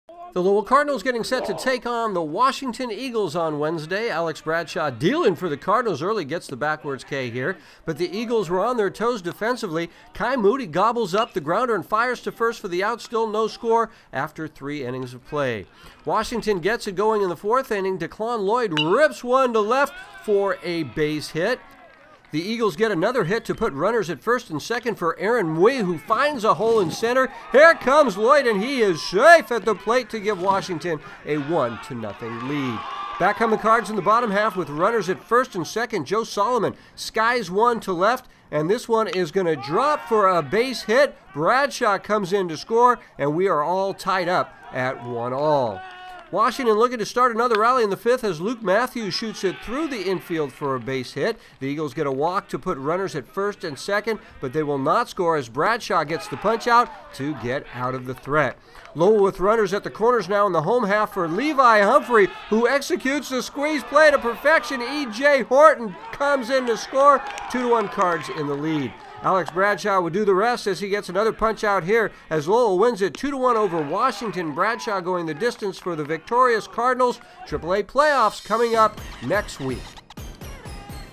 Washington at Lowell BB 043017
THE LOWELL CARDINALS GETTING SET TO TAKE ON THE WASHINGTON EAGLES ON WEDNESDAY

ALEX BRADSHAW WAS DEALING FOR THE CARDINALS EARLY...HE GETS THE BACKWARDS K HERE.

BUT THE EAGLES WAS ON THEIR TOES DEFENSIVELY...KAI MOODY GOOBLES UP THE GROUNDER AND FIRES TO FIRST FOR THE OUT, STILL NO SCORE AFTER 3 INNINGS.

WASHINGTON GETS IT GOING IN THE FOURTH...DECLAN LLOYD RIPS ONE TO LEFT FOR A SINGLE.

THE EAGLES GET ANOTHER HIT TO PUT RUNNERS AT FIRST AND SECOND FOR AARON MUI WHO FINDS THE HOLE IN CENTER...HERE COMES LLOYD AND HE IS SAFE AT THE PLATE TO GIVE WASHINGTON A 1-0 LEAD.

BACK COME THE CARDS IN THE BOTTOM HALF...WITH RUNNERS AT FIRST AND SECOND JOE SOLOMON SKIES ONE TO LEFT, THAT DROPS FOR A BASE HIT...BRADSHAW SCORES TO TIE IT UP AT 1 ALL.

WASHINGTON LOOKING TO START ANOTHER RALLY IN THE FIFTH AS LUKE MATTHEWS SHOOTS IT THROUGH THE INFIELD FOR A HIT

THE EAGLES GET A WALK TO PUT RUNNERS AT FIRST AND SECOND BUT THEY WILL NOT SCORE AS BRADSHAW GETS THE PUNCHOUT TO GET OUT OF IT.

LOWELL WITH RUNNERS AT THE CORNERS IN THE HOME HALF FOR LEVI HUMPHREY WHO EXECUTES THE SQUEEZE TO PLATE EJ WHORTON, 2-1 CARDS IN THE LEAD.

ALEX BRADSHAW WOULD DO THE REST AS HE GETS ANOTHER PUNCHOUT HERE AS  LOWELL WINS IT 2-1 OVER WASHINGTON. BRADSHAW GOING THE DISTANCE FOR THE VICTORIOUS CARDINALS.